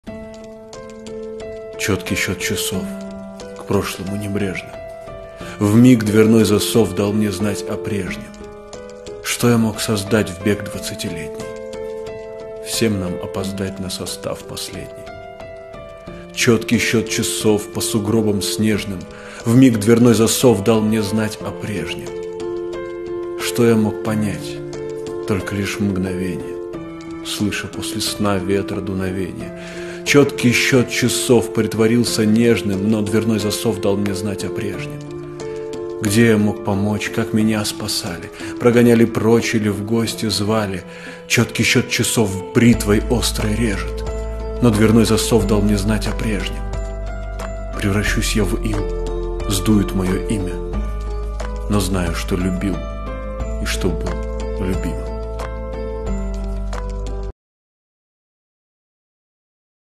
Чёткий счёт часов | Читает автор: Игорь Бушмелев
Чёткий счёт часов -
К прошлому небрежно.
Вмиг дверной засов
Дал мне знать о прежнем.

Что я смог создать
В бег двадцатилетний?
Всем нам опоздать
На состав последний.

Чёткий счёт часов -
По сугробам снежным.

Что я смог узнать -
Только лишь мгновенья,
Слыша после сна
Ветра дуновения.

Чёткий счёт часов
Притворился нежным,
Но дверной засов

Как я смог помочь,
Где меня спасали?
Прогоняли прочь
Или в гости звали?

Чёткий счёт часов
Бритвой острой режет,
Вновь дверной засов

Превращусь я в ил,
Сдует моё имя.
Знаю, что любил
И что был любимым.